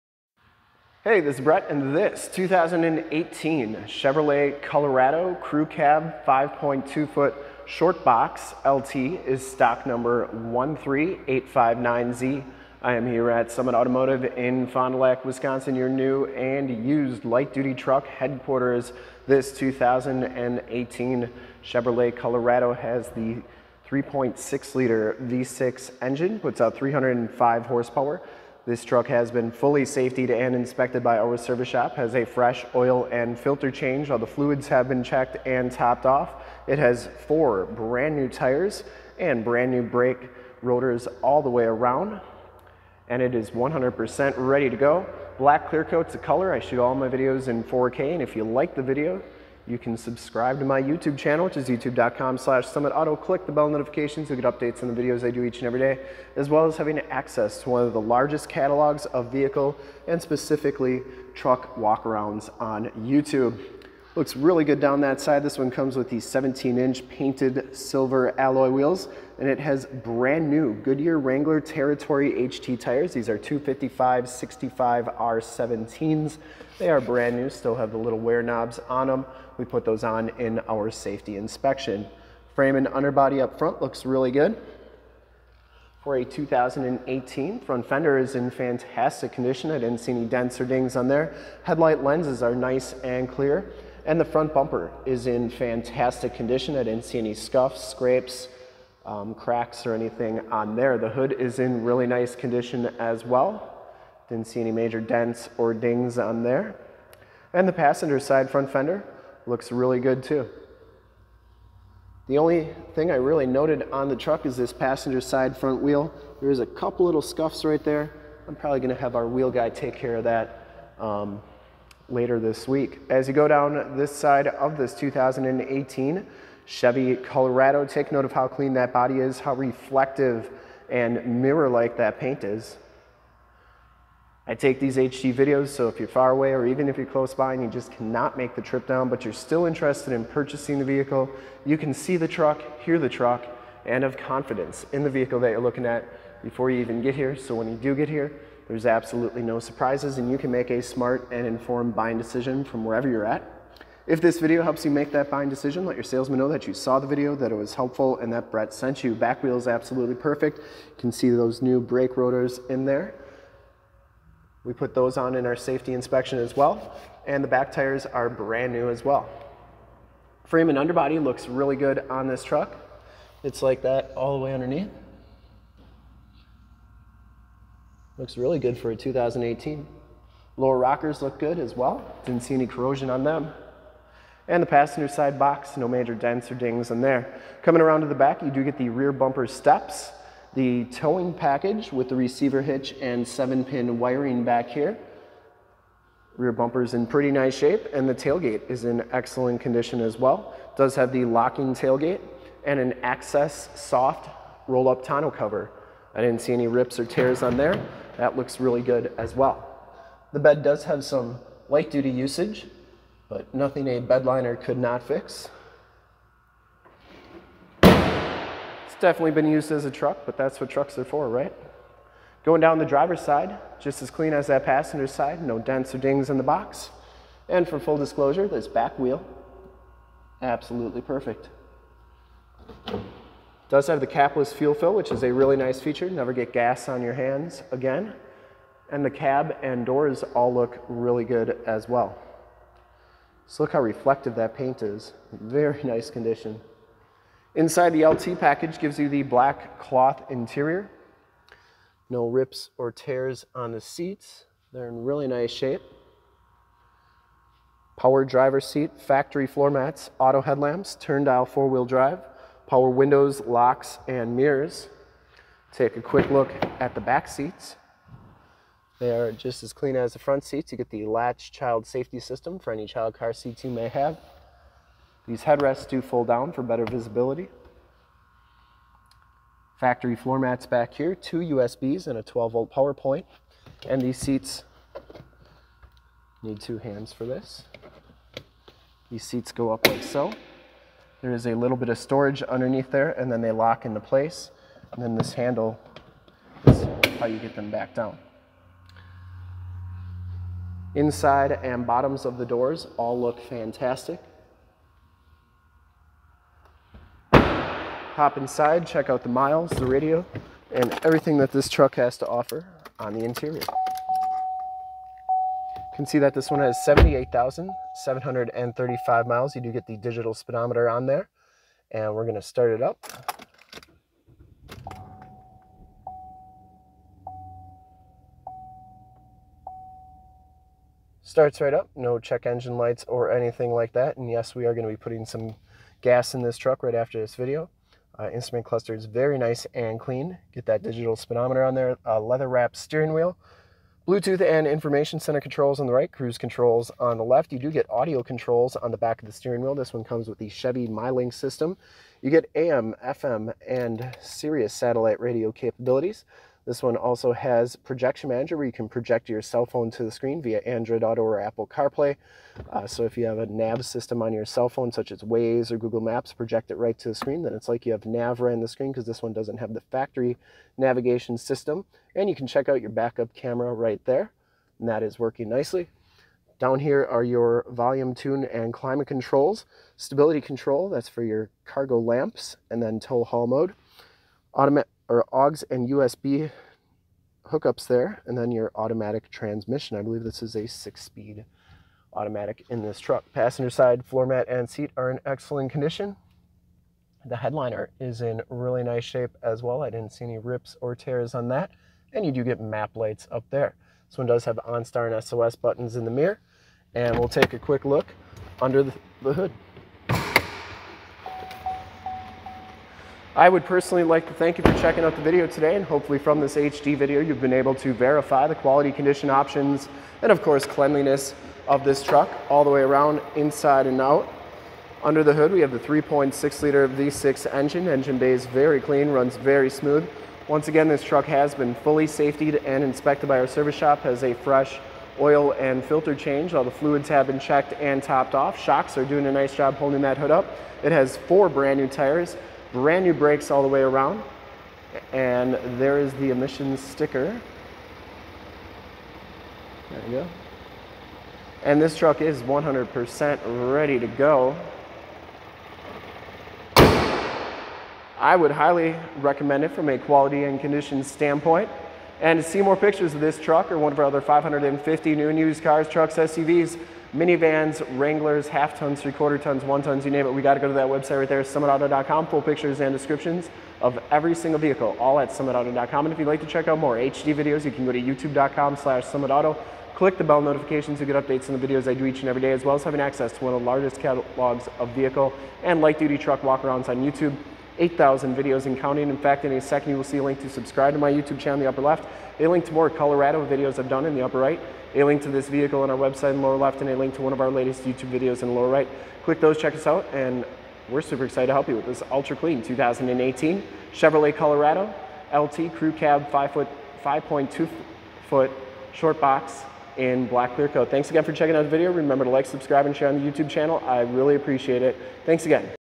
2018 CHEVROLET COLORADO CREW SHORT LT BLACK CLEARCOAT 4K WALKAROUND 13859Z SOLD!
This 2018 CHEVROLET COLORADO LT CREW SHORT IN BLACK CLEARCOAT FOR SALE IN FOND DU LAC OSHKOSH WISCONSIN 54935 is the vehicle we did walk around review of today.

Thank you for checking out this video of this 2018 CHEVROLET COLORADO LT CREW SHORT IN BLACK CLEARCOAT FOR SALE IN FOND DU LAC OSHKOSH WISCONSIN 54935

STOCK: 13859Z
PRICE: $26,991
MILES: 78,722
MAKE: CHEVROLET
MODEL: COLORADO
VIN: 1GCGTCEN1J1296315
LOCATION: FOND DU LAC OSHKOSH WISCONSIN, 54937 TRUCKS ON 41

CLEAN TITLE HISTORY! CLEAN CARFAX! 3.6 Liter DOHC V6 Engine, 305 Horsepower, Full Four Door Crew Cab, Short Box 5.2 Foot Shortbox, LT Package, Automatic Transmission Center Console Shifter, Turn Dial 4x4 Four Wheel Drive 4WD, Reverse Backup Camera Rearview Camera, Onstar System, Power Driver's Seat, Black Ebony Cloth Seats, Bucket Seats, Access Soft Rollup Tonneau Cover, Towing Package with Receiver Trailer Hitch and Trailer Wiring Tow Package, Power Mirrors, Stabilitrak Traction Control, P255/65 R17 Tires , Painted Alloy Rims Premium Wheels, Four Wheel Disc Brakes, Bedrail Covers, Locking Tailgate, Rear Bumper Steps, Chrome Trimmed Grill, Chevy MyLink Touchscreen Radio, AM / FM Radio Tuner, Sirius/XM Satellite Radio Capabilities Sirius / XM, Bluetooth, Hands-Free Phone Controls Blue Tooth, Auxiliary MP3 Jack Portable Audio Connection, USB Jack Portable Audio Connection, Keyless Entry System, Adjustable Height Seatbelts, Driver and Passenger Front Air Bags, L.A.T.C.H. Child Safety System, Side Curtain Air Bags SRS Safety Restraint System, Multi-Function Steering Wheel Controls, Compass, Outside Temperature Display and Mileage Display, Factory Floormats, Air Conditioning AC, Cruise Control, Power Locks, Power Windows, Automatic Headlights Autolamp, Tilt/Telescope Steering Wheel, Black, CLEAN AUTOCHECK! Very very clean inside and out! This is one of the sharpest 2018 Chevrolet Colorado 4wd trucks on our lot! Make your move before this super clean 4wd is gone!

STOCK: 13859Z
PRICE: $26,991
MILES: 78,722
MAKE: CHEVROLET
MODEL: COLORADO
VIN: 1GCGTCEN1J1296315